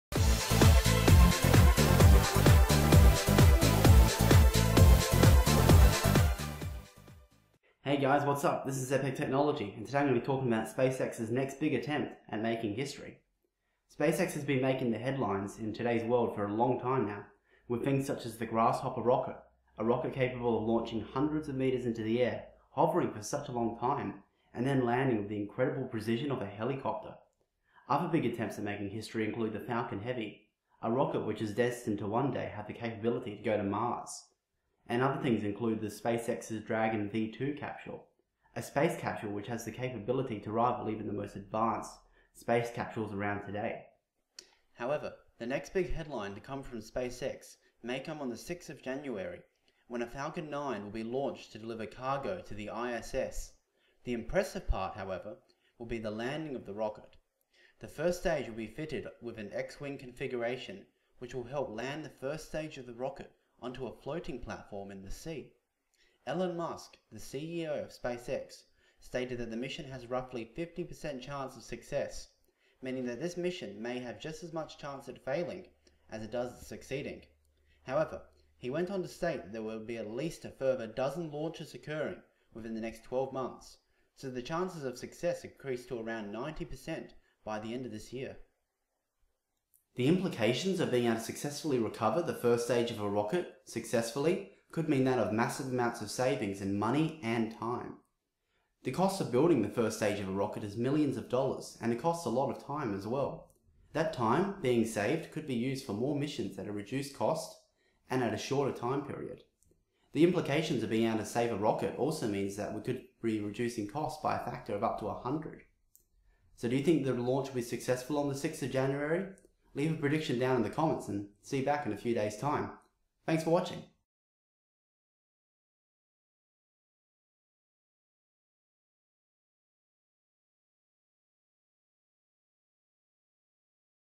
SpaceX to make historic spaceflight attempt this week
SpaceX is set to attempt a spaceflight which will involve the recovery of the first stage of the rocket, what implications does this have if the launch is successful?